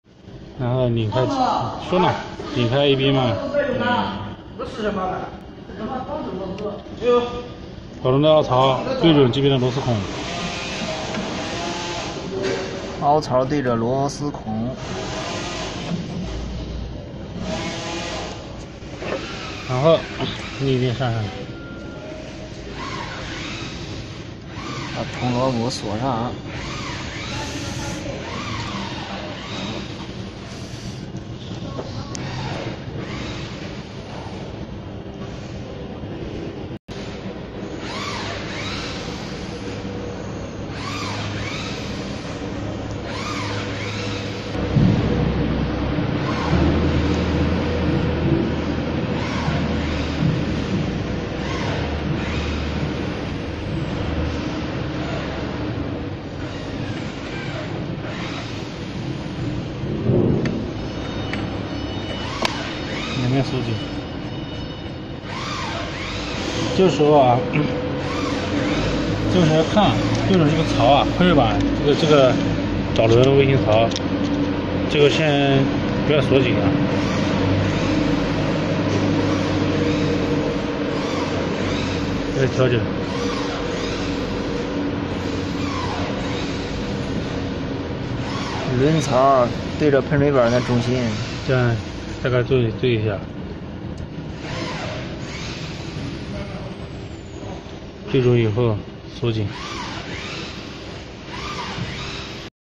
Steps for changing the guide pulleys for Wire cut EDM machine
The wire guide wheels for Reusable wire cut machine, Molybdenum wire cutting machine are consumption parts. Every 4-6 months please check it,if not running smoothly, please change it.